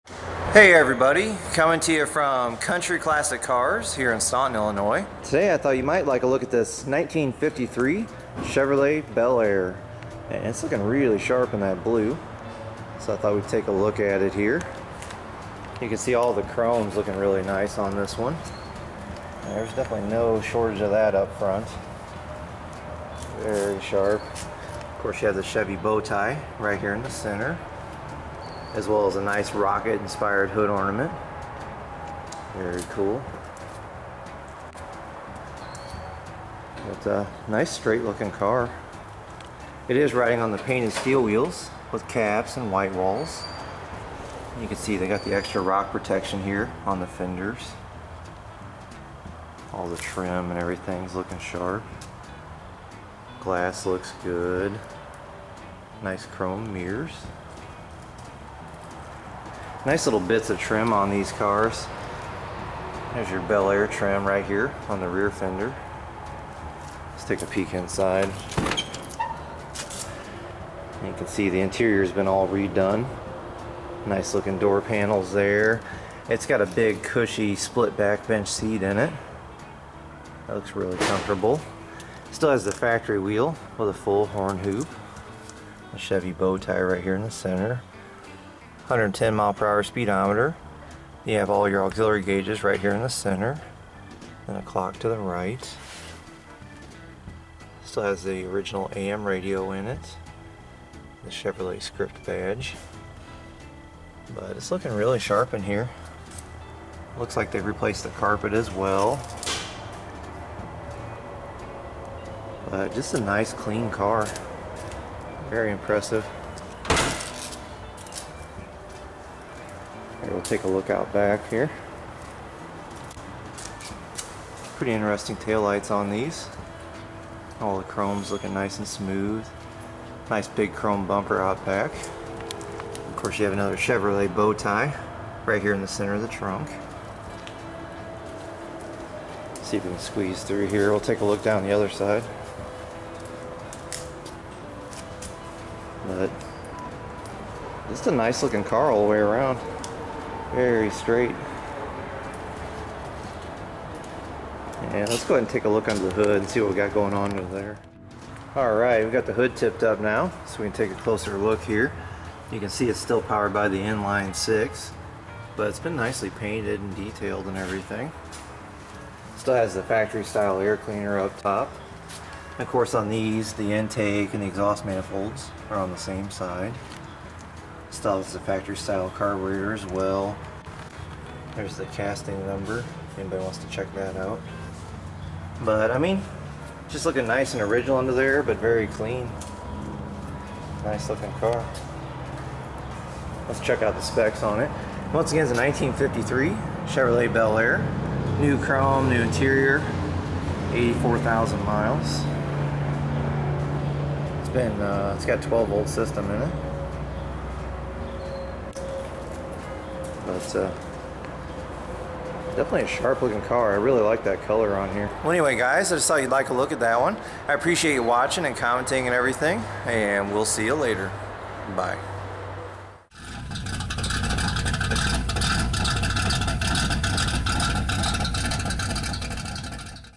1953 Chevrolet Bel Air Two Door - Very Nice at Country Classic Cars
I thought you might like a look at this nicely restored 1953 Chevrolet Bel Air 2 dr.  It looks really sharp in that blue exterior and it has a custom interior as well.  It's powered by the inline six cylinder and runs great.  Like most cars of it's time, it has tons of chrome everywhere you look.  I hope you find it interesting, thanks for taking a look!